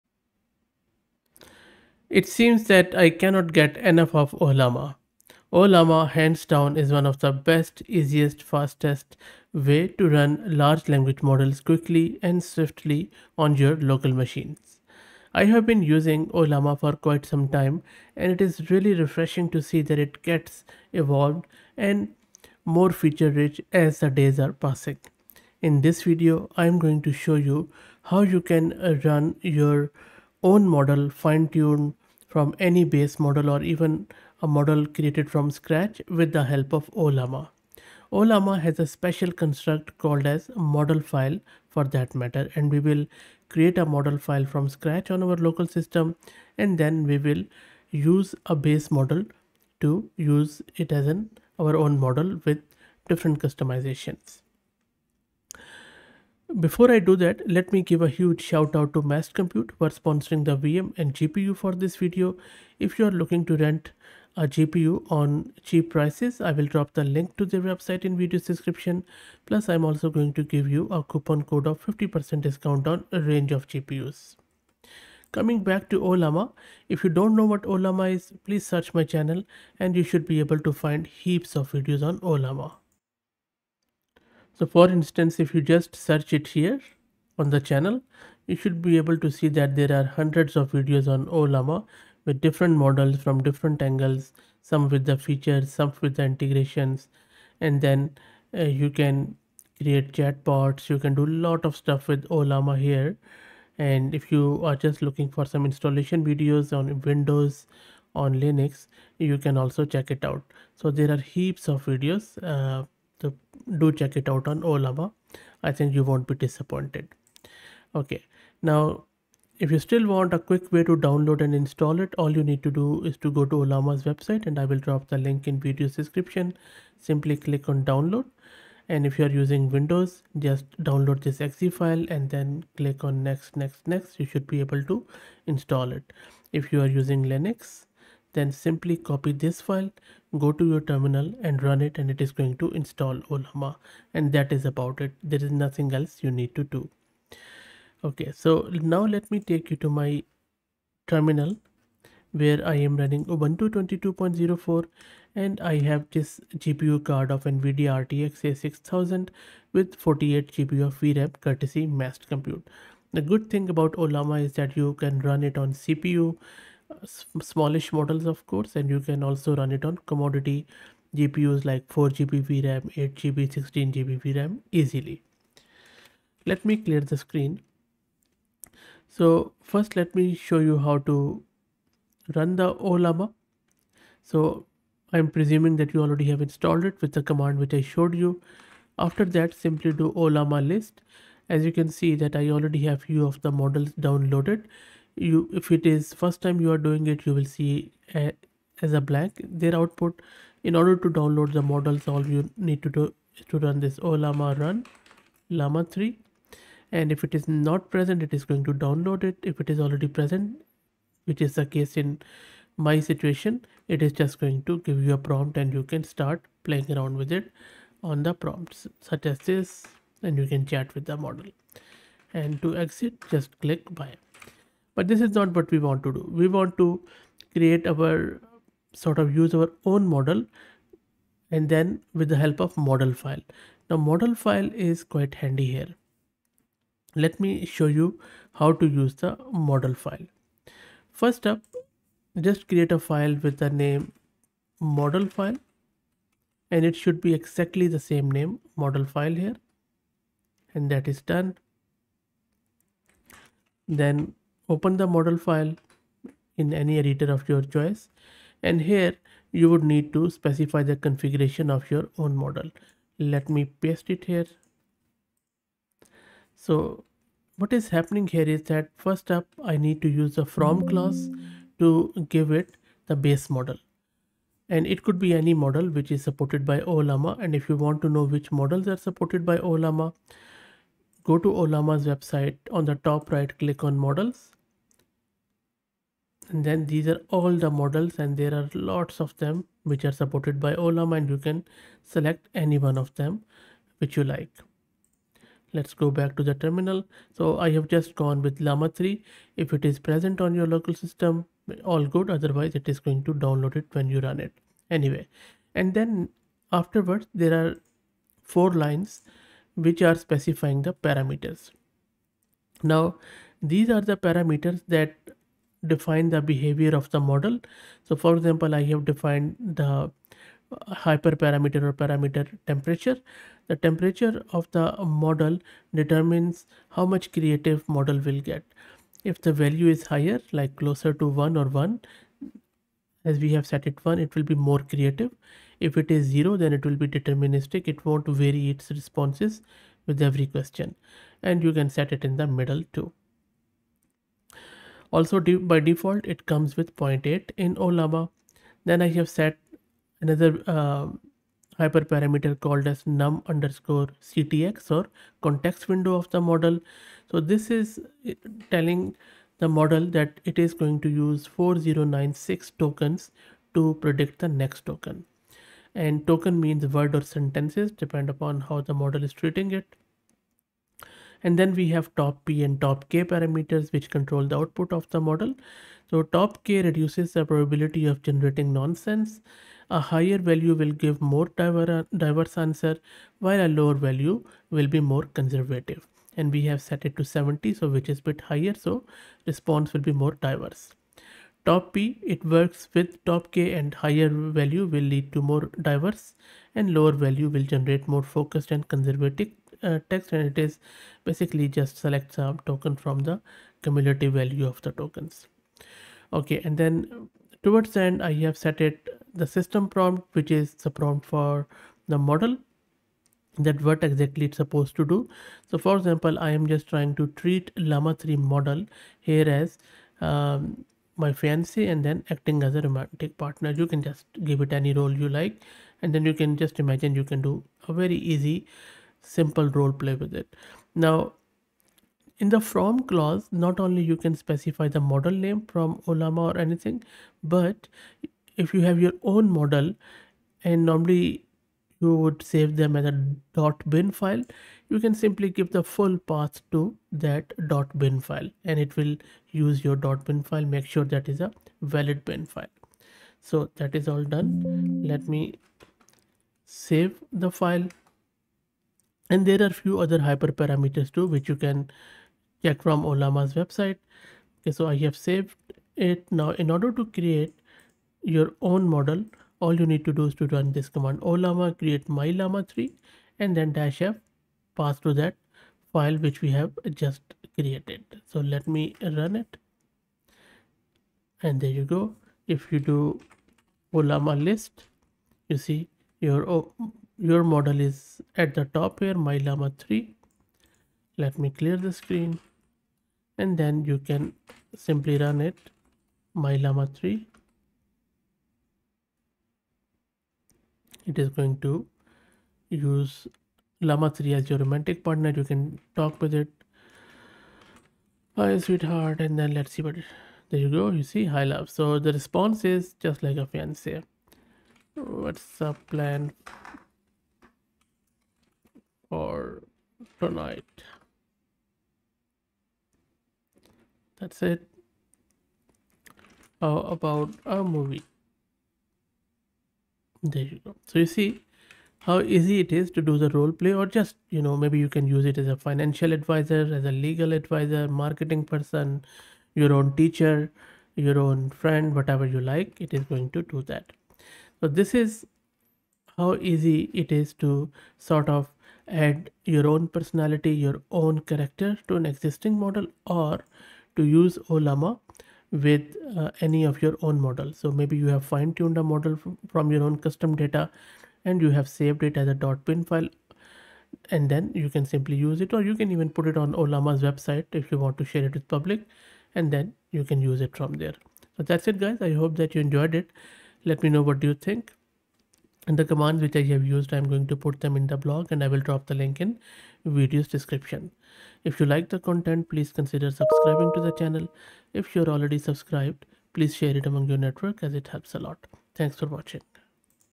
How to Use ModelFile in Ollama
This video is a step-by-step tutorial to create a ModelFile and customizing your own model with Ollama locally.